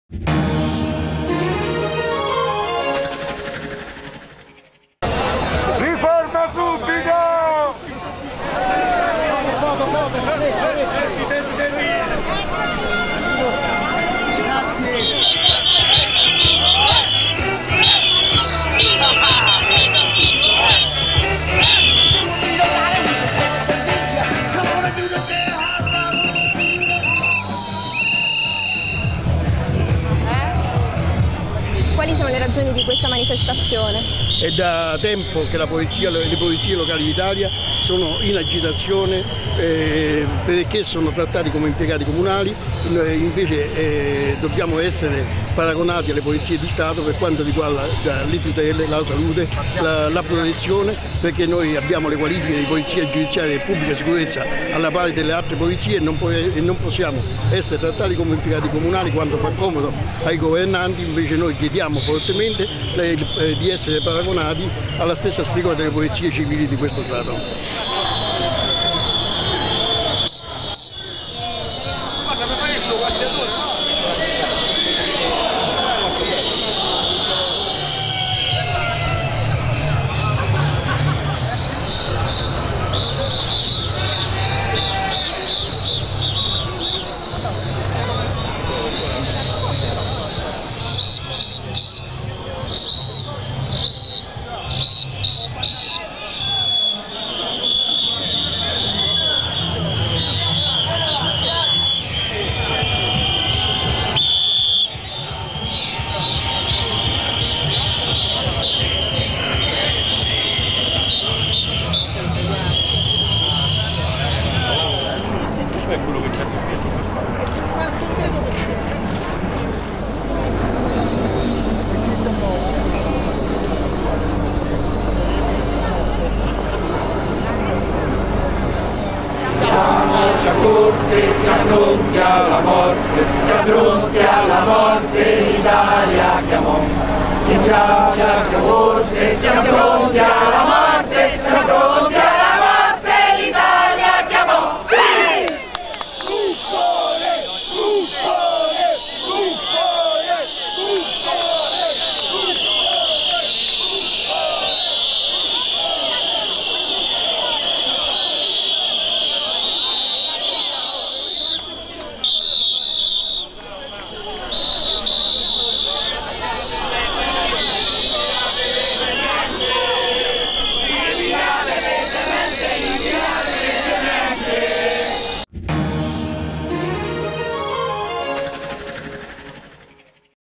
Vigili in protestano sfilano per il centro: Vogliamo subito una riforma
È arrivato a piazza Bocca della Verità il corteo dei vigili  d'Italia per lo sciopero nazionale della categoria, indetto dal Csa-Ospol e partito da piazza della Repubblica. Dal palco il segretario romano dell'Ospol, Stefano Lulli, ha attaccato: "Il Campidoglio e la sede del Comando generale sono blindati. Di cosa hanno paura, della vergogna che hanno?". Per Lulli "oggi ha vinto chi sta qui, non chi è rimasto a casa o al lavoro. Voi avete vinto!". E racconta: "Avete visto la vigliaccata del Comune di Roma che solo a poco più di 24 ore dalla manifestazione ci ha mandato il permesso necessario. Speravano che mollassimo e invece siamo andati avanti, ce l'abbiamo fatta". Sono previsti ora interventi di agenti arrivati da varie città della Penisola.